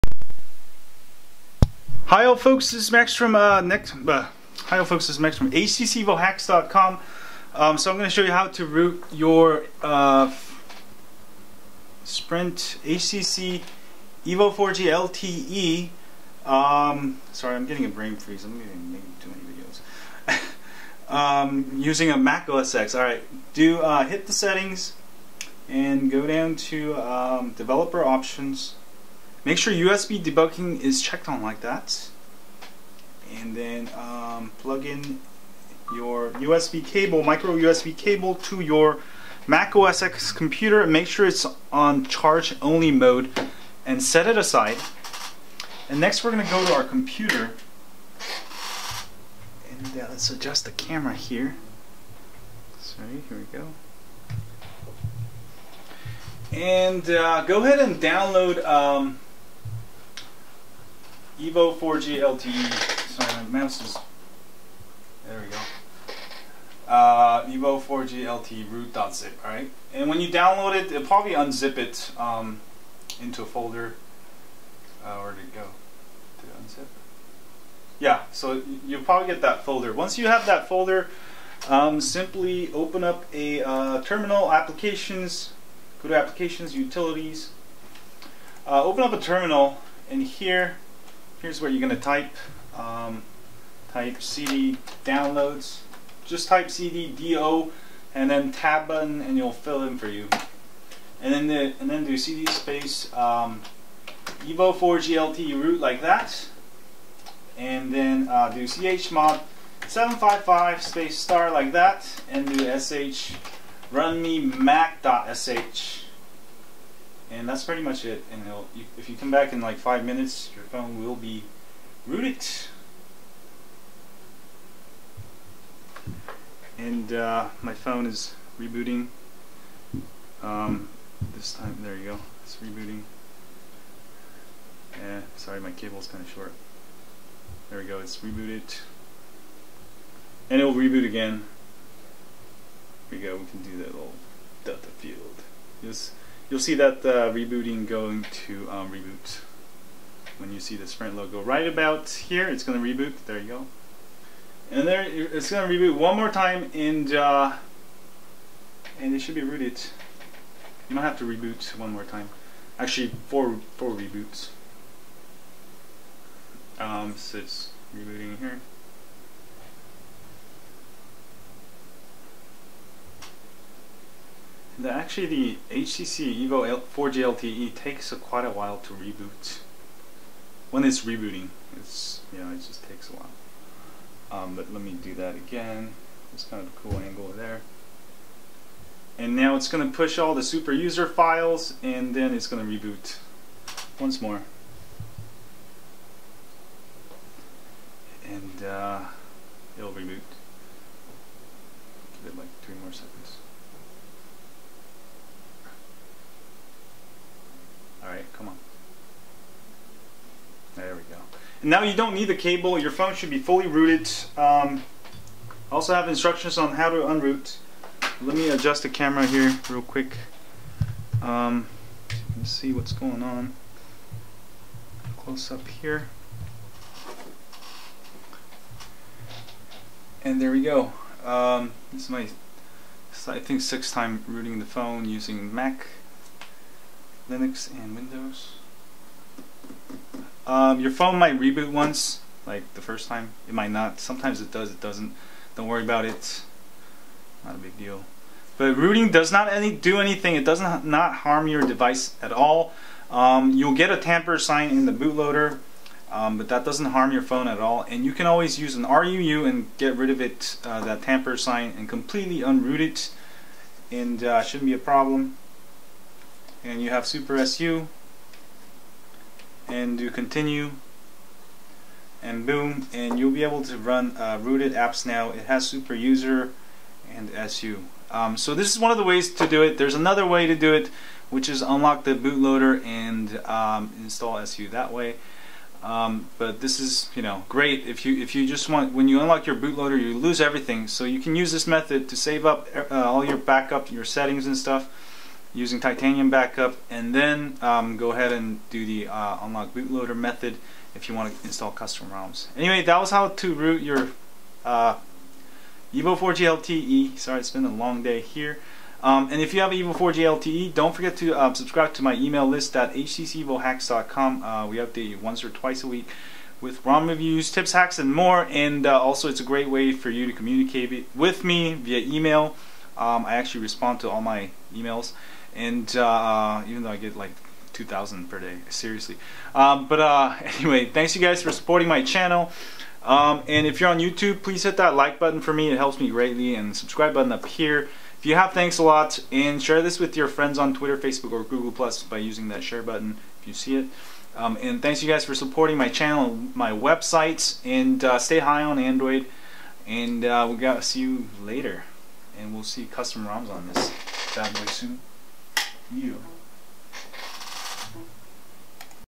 How to root your HTC Evo 4G LTE on Mac OSX!
Here's how to root your HTC Evo 4G LTE using Mac OSX!

Also works on all Windows, Mac and Linux!

Want to stay updated on latest HTC Evo 4G LTE hacks and tips?